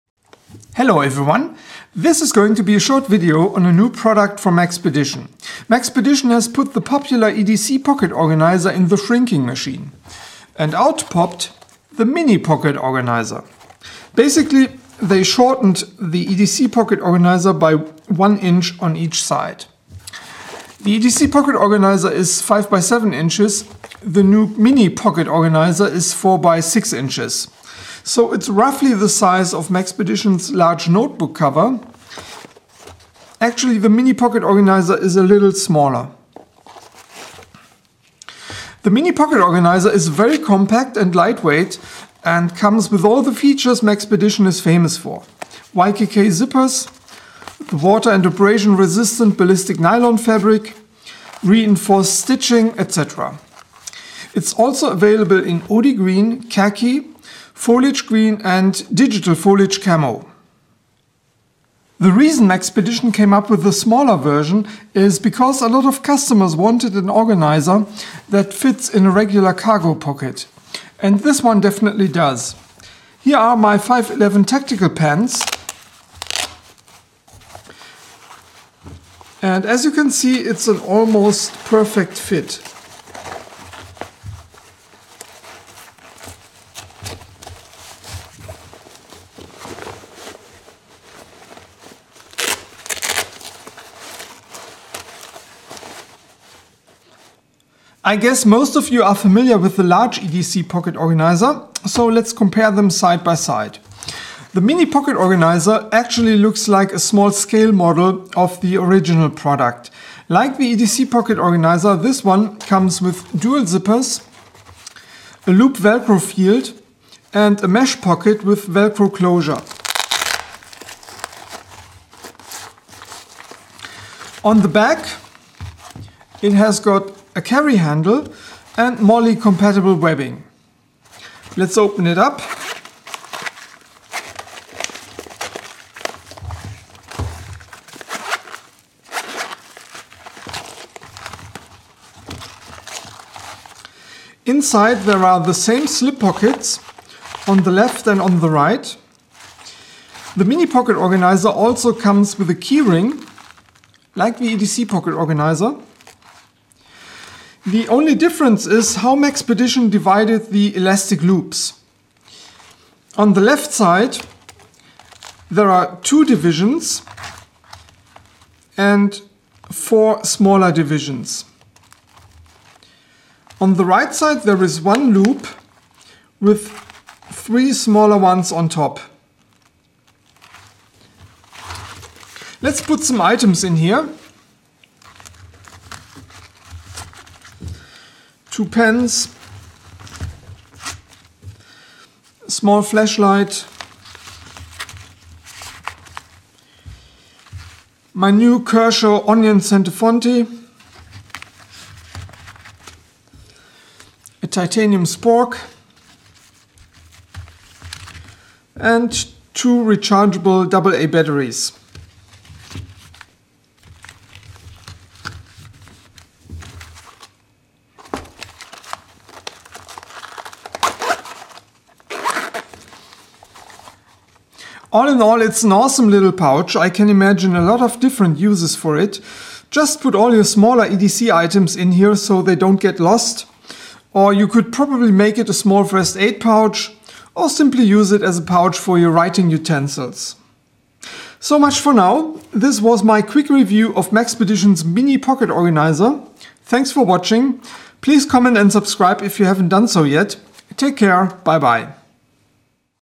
Maxpedition Mini Pocket Organizer
This is a quick review of Maxpedition's new Mini Pocket Organizer, a smaller version of the popular EDC Pocket Organizer.